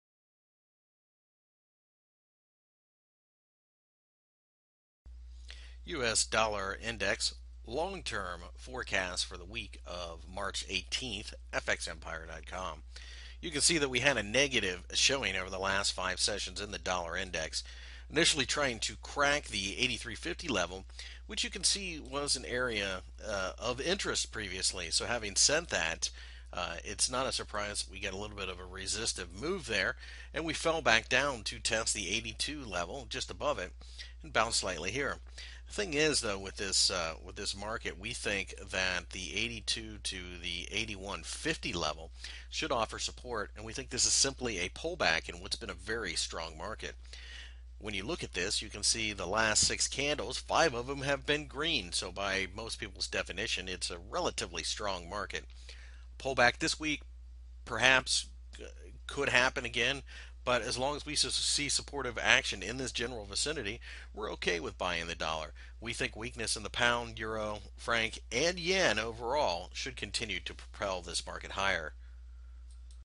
US Dollar Week Forecast for the week of March 18, 2013, Technical Analysis
- March 18, 2013 index weekly technical analysis for the US Dollar INDEX.Find more information about Forex and Commodities News